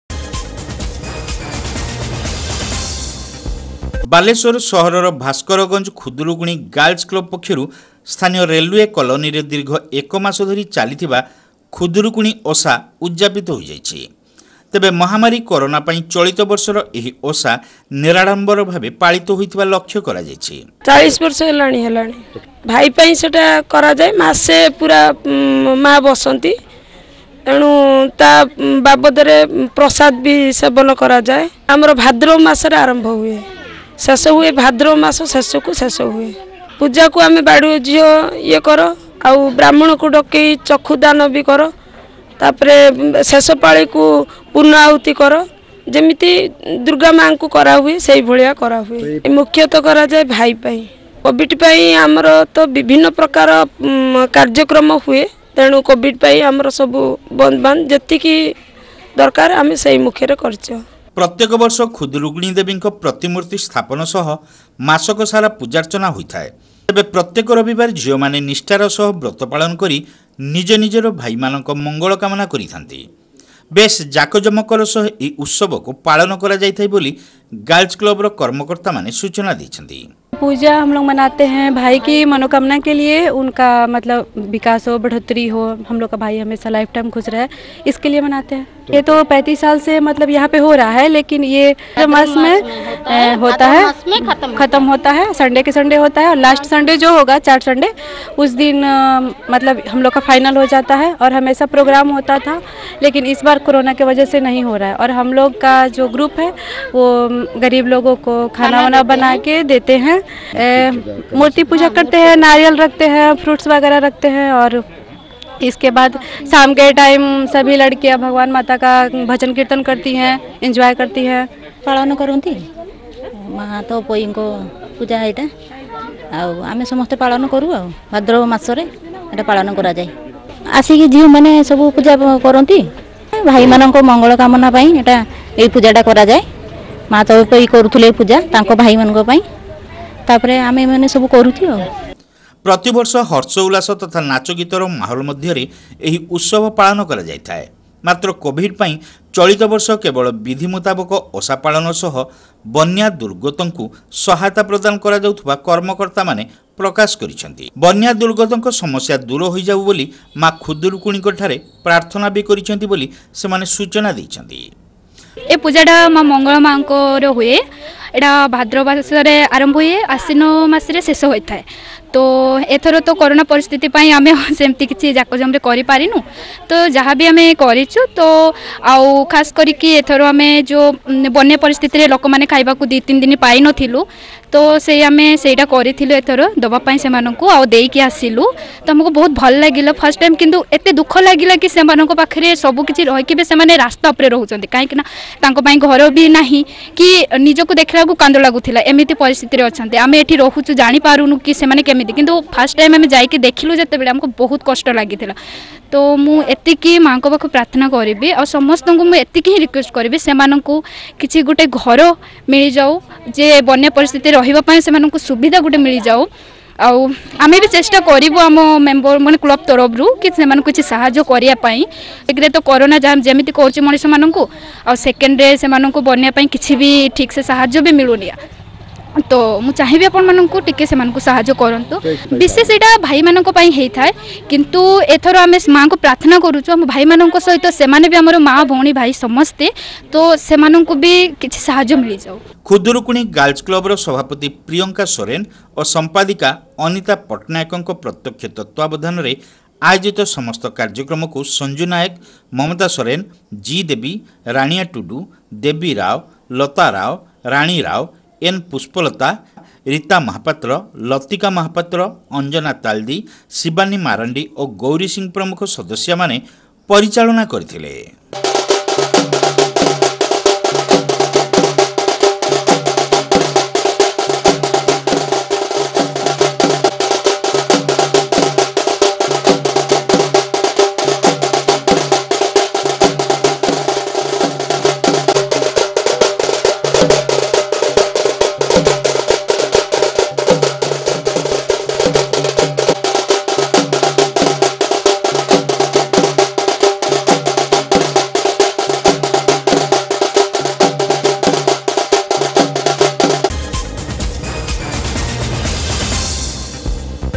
ମାଧବ ନ୍ୟୁଜ - ଉଦଯାପିତ ହେଲା ରେଲୱେ କଲୋନୀ ଗାର୍ଲ୍ସ କ୍ଲବର ଖୁଦୁରୁକୁଣୀ ଓଷା
Madhab News - Khudurkuni Festival Organised by Balasore Railway Colony Girls Club Concluded - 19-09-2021